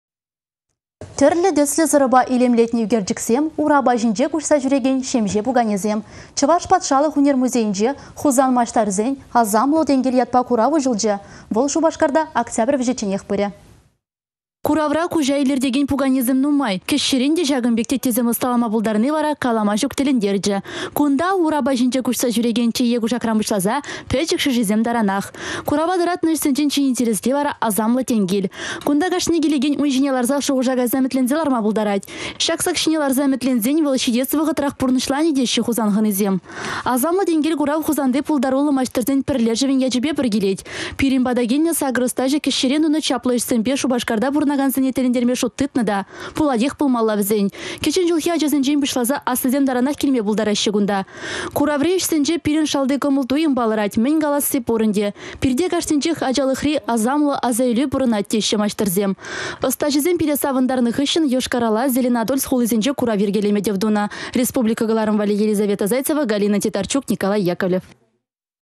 Шупашкарта асамлă сак çинче ĕмĕтленме пулать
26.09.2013
Тĕрлĕ-тĕслĕ сăрăпа илемлетнĕ ÿкерчĕксем, урапа çинче  куçса çÿрекен  çемçе  пуканесем...Чăваш патшалăх ÿнер музейĕнче Хусан маçтăрĕсен «Асамлă тенкел» ятпа курав уçăлчĕ. Вăл Шупашкарта октябрь вĕçĕчченех пырĕ.